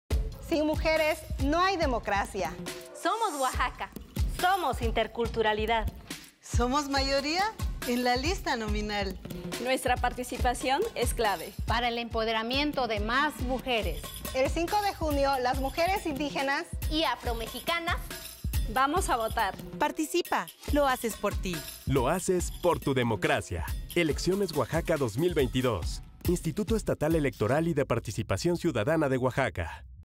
Promoción del Voto Mujeres Indígenas y Afromexicanas
El domingo 5deJunio todas las mujeres tienen derecho a votar de manera libre, secreta y directa. Todas las personas debemos vigilar que las oaxaqueñas ejerzan su derecho al voto sin ningún impedimento. Sin mujeres no hay democracia. ¡Vota, hazlo PorTuDemocracia!